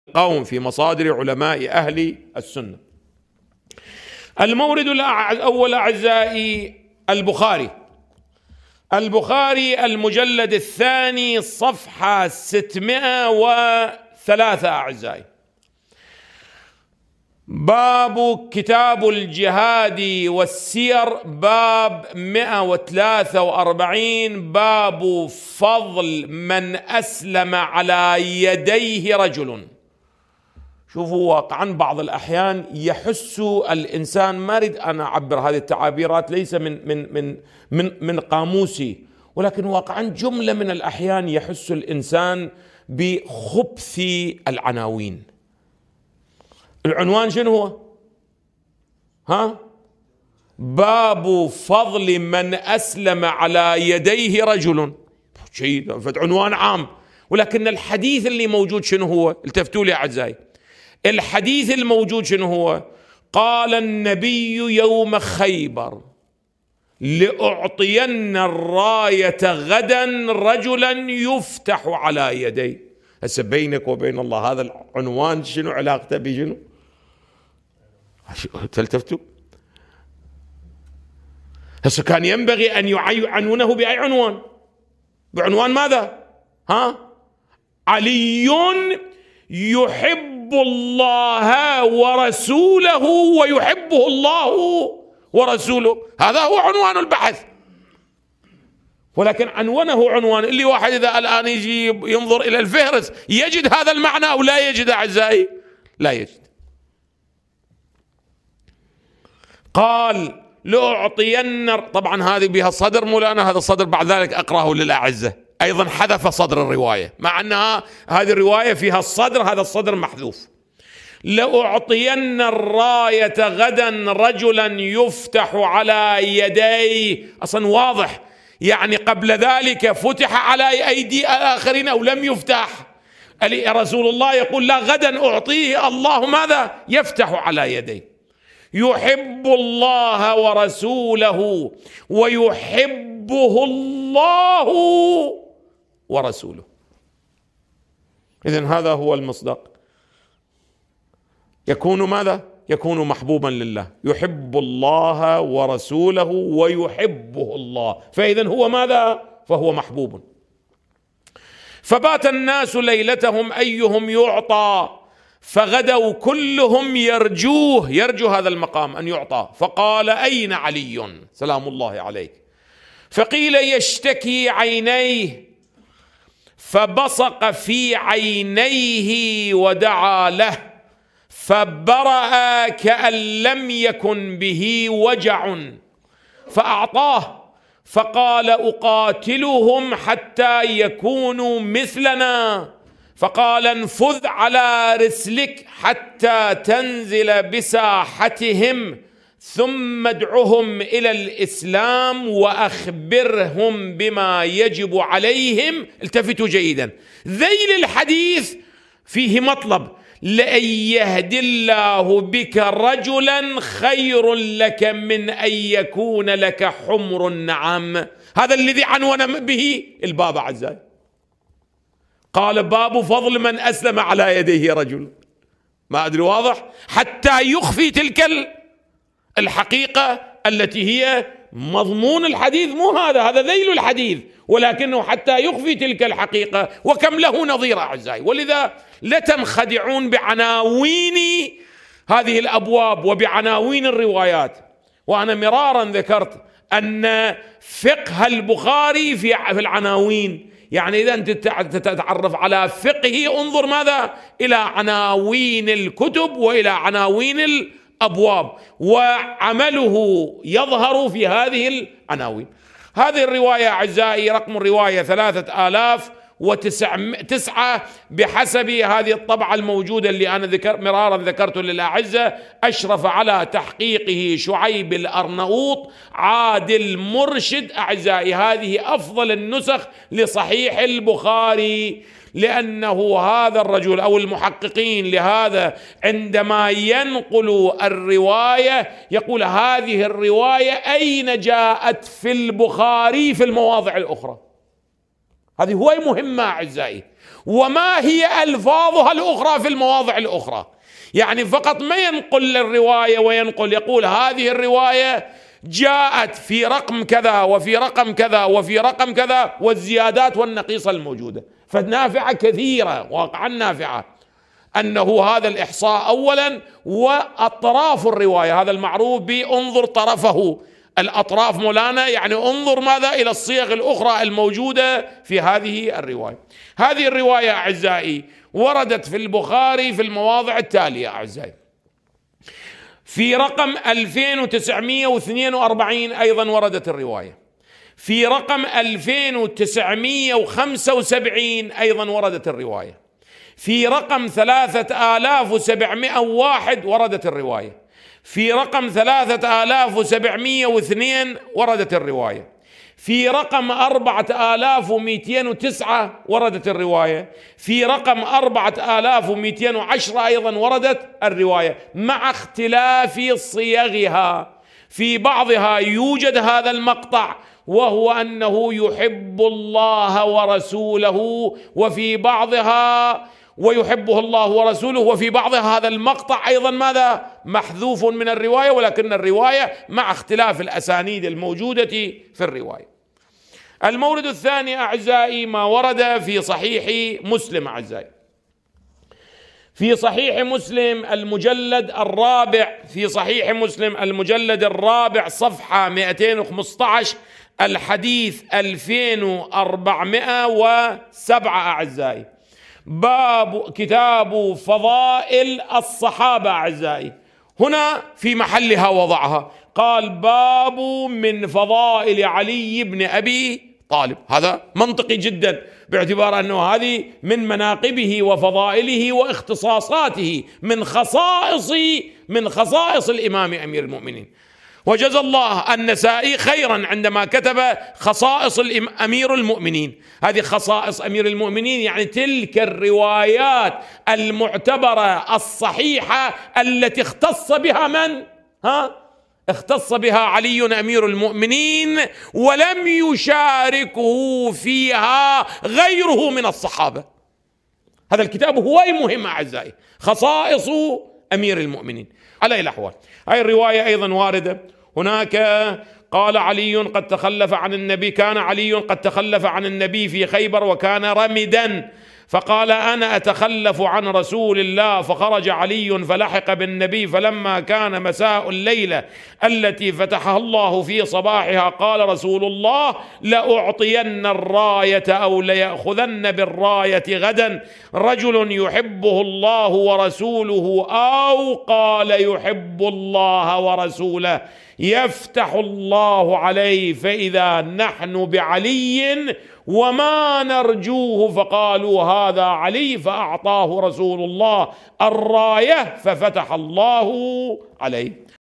مصادر حديث (الامام علي يحبه الله و رسوله) عند أهل السنة | السيد كمال الحيدري
عنوان الدرس:السّنة النبوية موقعها، حجيتها، أقسامها 65
المورد الأول: البخار المجلد الثاني صفحة 603 باب كتاب الجهاد والسير باب 143 باب فضل من اسلم على يديه رجلٌ انظروا واقعاً في بعض الاحيان يحس الانسان واقعاً انا لا اريد أن اعبر بعض هذه التعابير ليس من قاموسي ولكن واقعاً جملة من الاحيان يحس الانسان بخبث العناوين، العنوان باب فضل من اسلم على يديه رجلٌ عنوان عام ولكن الحديث الموجود ما هو؟ قال النبي يوم الخيبر لاعطين الراية غداً رجلاً يفتح على يديه.

الآن بينك وبين الله هذا العنوان ما علاقته به؟ هذا كان ينبغي أن يعنونه بأي عنوان؟ بعنوان علي يحب الله ورسوله ويحبه الله ورسوله هذا هو عنوان البحث ولكن عنونه الذي واحد الان ياتي ينظر الى الفهرست يجد هذا المعنى أو لا يجد؟ لا يجد وطبعاً هذا فيه صدر وبعد ذلك سأقرأها اليكم أيضاً حذف صدر الرواية مع هذا أن هذه الرواية لها صدر والصدر محذوف، لاعطين الراية غدا رجل يفتح على يديه اصلا واضح يعني قبل ذلك فتح على أيدي الاخرين أو لم يفتح؟ رسول الله يقول لا غداً اعطيه الله يفتح على يديه يحب الله ورسوله ويحبه الله ورسوله.

اذن هذا هو المصداق يكون محبوباً لله يحب الله ورسوله ويحبه الله فاذن هو محبوبٌ فبات الناس ليلتهم ايهم يعطى فغدوا كلهم يرجوه فقال أين عليٌ سلام الله عليك فيقل يشتكي عينيه فبصق في عينيه ودعى له فبرئ كأن لم يكن به وجعٌ فاعطاه فقال اقاتلهم حتى يكونوا مثلنا فقال انفذ على رسلك حتى تنزل بساحتهم ثم ادعهم الى الاسلام واخبرهم بما يجب عليهم ذيل الحديث فيه مطلب لان يهدي الله بك رجلاً خير لك من أن يكون لك حمرٌ اعم هذا الذي عنون به الباب قال باب فضل من اسلم عليه رجلٌ حتى يخفي تلك الحقيقة التي هي مضمون الحديث ليس هذا، هذا ذيل الحديث ولكنه حتى يخفي تلك الحقيقة وكم له نظير ولذا لا تنخدعون بعناوين هذه الابواب وعناوين هذه الروايات.

وانا ذكرت مراراً أن فقه البخاري في العناوين يعني اذا تريد أن تتعرف على الفقه انظر الى عناوين الكتب والى عناوين الابواب واظهروا في هذه العناوين رقم الرواية 3009 بحسب هذه الطبعة الموجودة التي انا مراراً ذكرت لكم اشرف على تحقيقه شعيب الارنئوط عادل مرشد هذه افضل النسخ لصحيح البخاري لانه هذا الرجل أو المحققين بهذا عندما ينقل الرواية يقول هذه الرواية أين جاءت في البخاري في المواضع الاخرى هذه كثير مهمة وما هي الفاظها الاخرى في مواضعها الاخرى يعني فقط ما ينقل الرواية وينقل ويقول هذه الرواية جاءت في رقم كذا وفي رقم كذا وفي رقم كذا والزيادات والنقيصة الموجودة فنافعة كثيرة انه هذا الاحصاء اولاً واطراف الرواية هذا المعروف بانظر طرفه واطراف يعني انظر الى الصيغ الاخرى الموجودة في هذه الرواية، هذه الرواية وردت في البخاري في موارد متعددة في رقم 2942 أيضاً وردت رواية في رقم 2975 أيضاً وردت الرواية في رقم 3701 وردت الرواية، في رقم 3702 وردت الرواية، في رقم 4209 وردت الرواية، في رقم 4210 أيضاً وردت الرواية مع اختلاف صيغها في بعضها يوجد هذا المقطع وهو انه يحب الله ورسوله وفي بعضها هذا المقطع أيضاً محذوف من الرواية ولكن الرواية مع اختلاف الاسانيد الموجودة في الرواية.